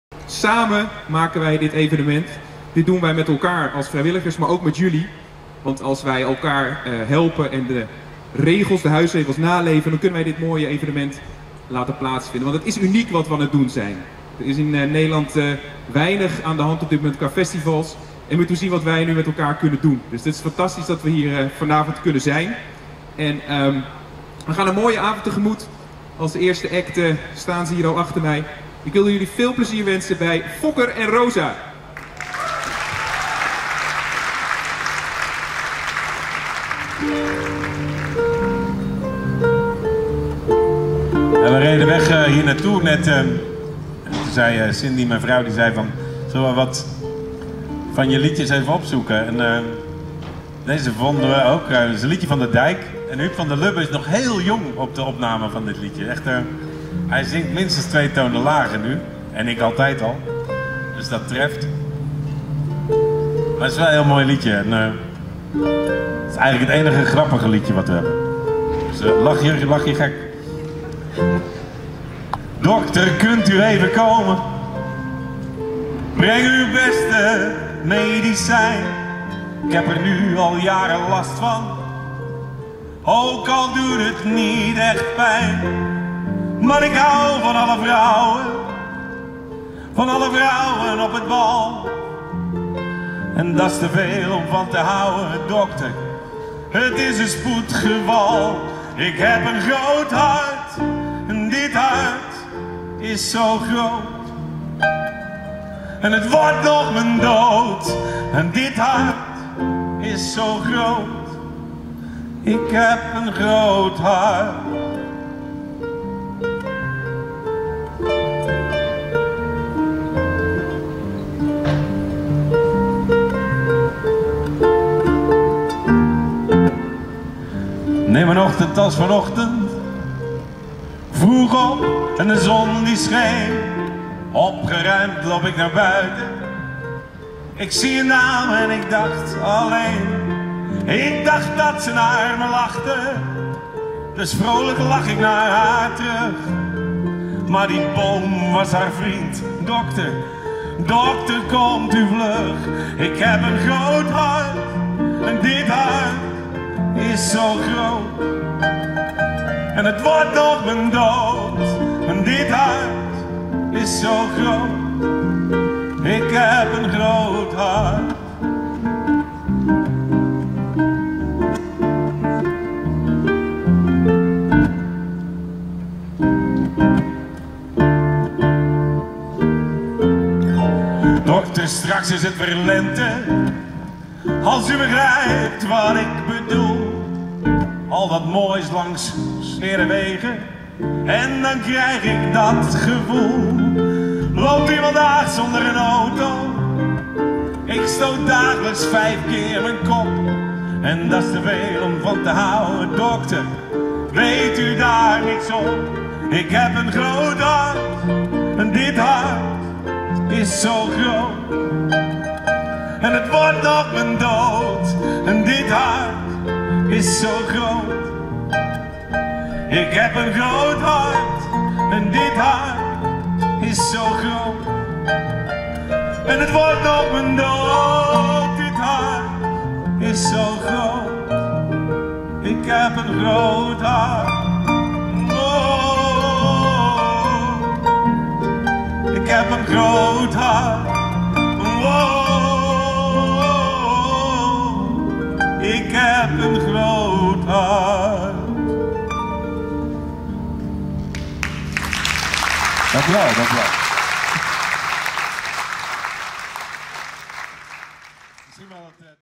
RIEtheater avond Fokker & Rosa 1
Optreden tijdens het Onder de bomen festival op het Excercitieveld in Woerden